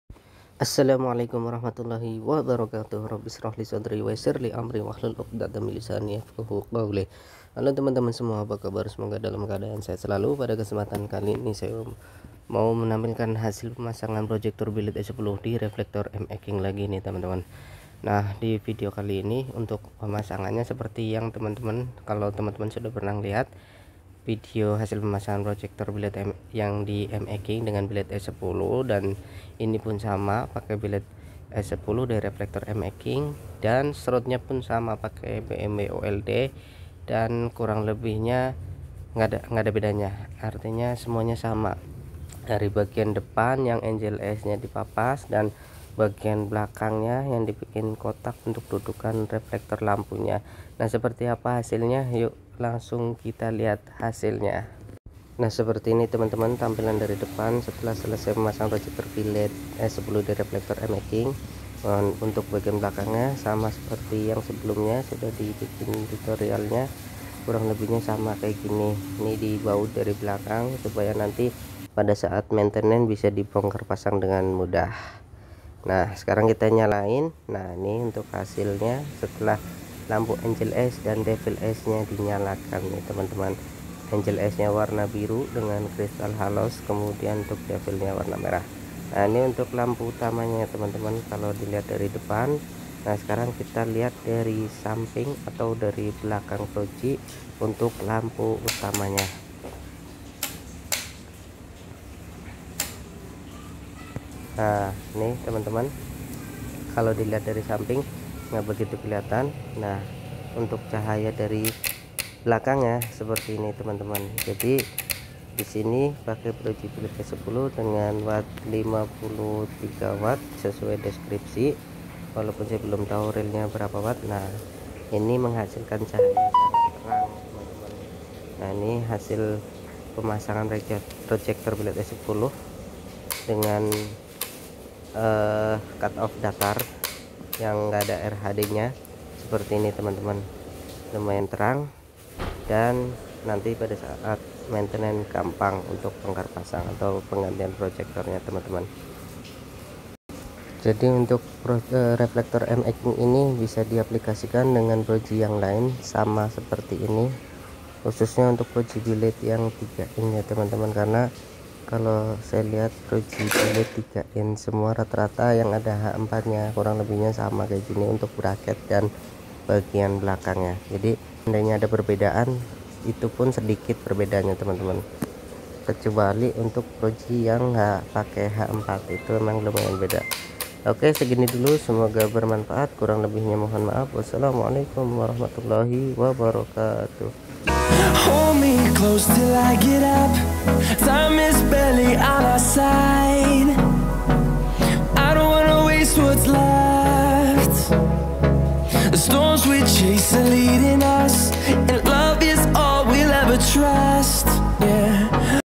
PROJECTOR E10 IN HEADLAMP MX KING 3INCH | Hadi Sutrisno
Assalamualaikum
Berikut kami sajikan vidio PROJECTOR E10 IN HEADLAMP MX KING 3INCH | Hadi Sutrisno, untuk lebih jelasnya proses pemasanganya bisa ditonto di vidio yang berjudul TUTORIAL PASANG PROJIE BILED E10 DI REFLEKTOR MX KING